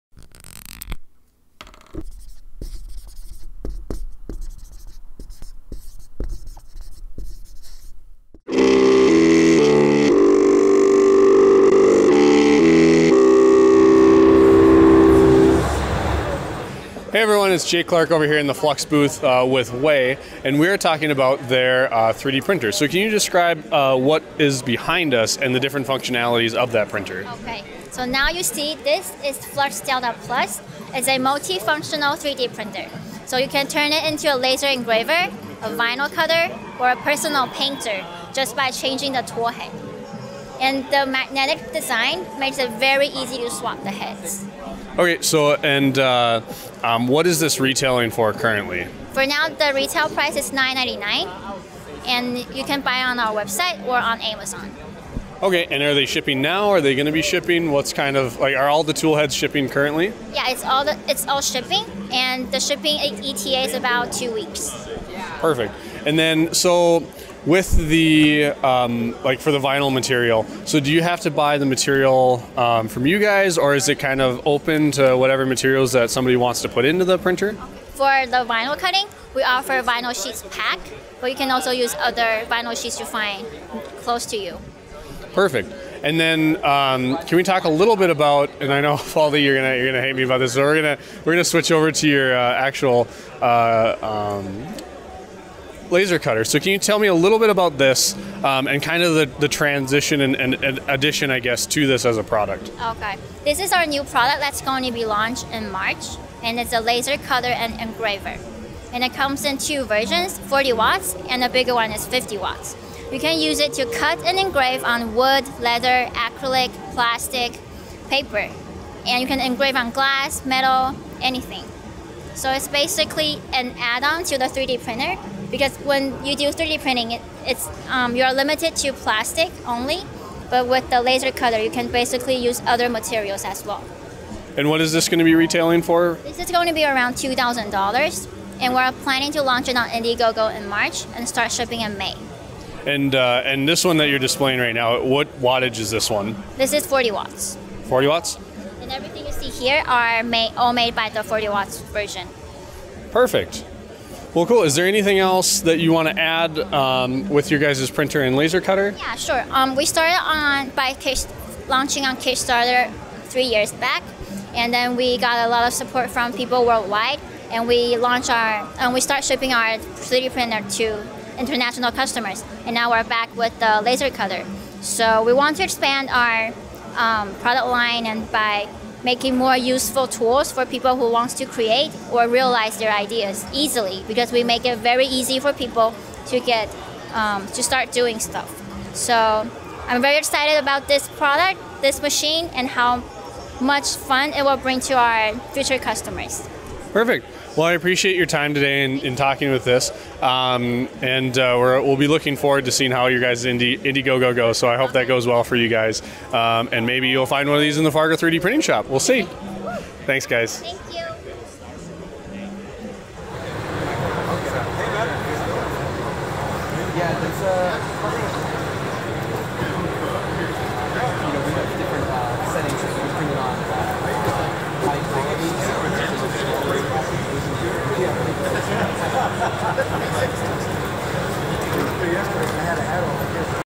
FLUX Delta+ Multi-Tool & Beambox Laser Cutter | CES 2018
FLUX probably has the strangest marketing video we've ever seen for a 3D printer. The printer is far from standard, so it fits pretty well.

With swappable magnetic heads, the FLUX Delta+ can 3D print, laser engrave, cut vinyl, scan and more (open SDK). This follows the success of FLUX Delta, which hit Kickstarter in 2014.
It's currently $999 on Amazon and their website with a two week shipping eta.

FLUX also showed off a CO2 laser cutter/engraver called the Beambox. It can work with wood, leather, acrylic, paper, glass and metal. There will be 40 and 50W versions, the latter featuring a slightly larger build area.
Beambox is coming to Indiegogo this March, shipping in May.